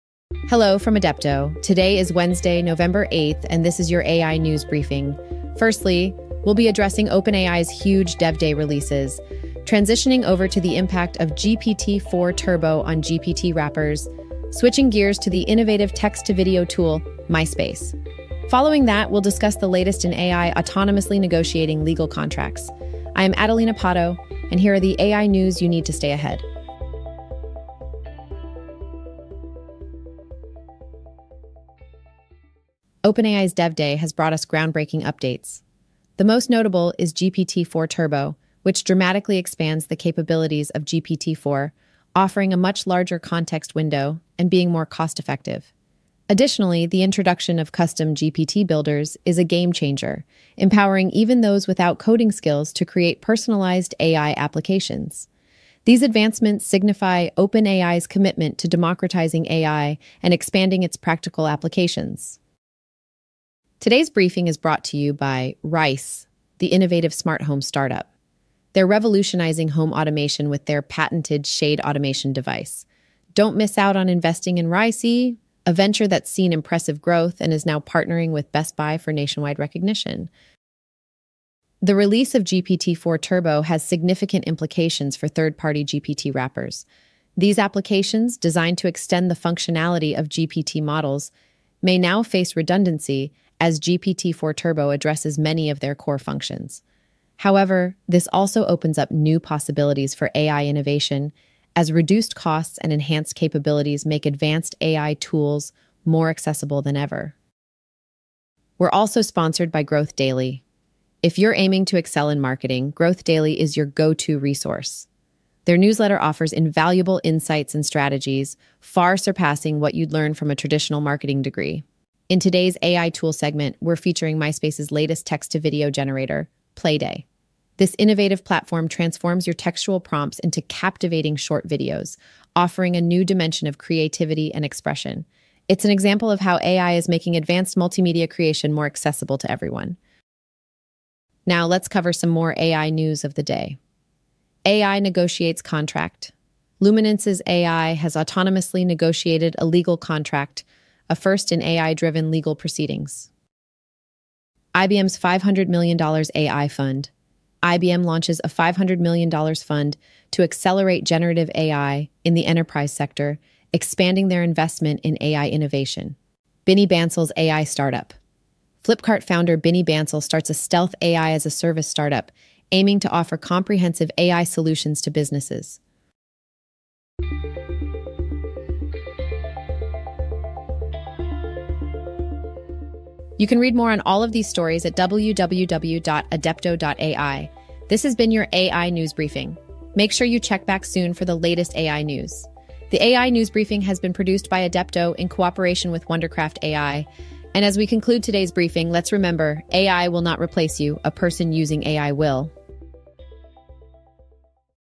OpenAI's DevDay Announcements & The Death For GPTWrappers | AI News Briefing | 08.11.2023
AI News Briefing - November 6, 2023

for a deeper dive into these fascinating developments and for the latest AI news and insights.

Timestamps:
(00:00) OpenAI's DevDay Reveals
(00:33) RYSE's Home Automation
(01:03) Impact of GPT-4 Turbo
(01:21) Growth Daily's Marketing Hacks
(02:02) PlaiDay's Text-to-Video Tech
(02:21) Luminance's Legal Contract Negotiation
(02:34) IBM's $500M AI Fund
(02:46) Binny Bansal's Next Venture

For creating our podcast episodes, we use Wondercraft AI.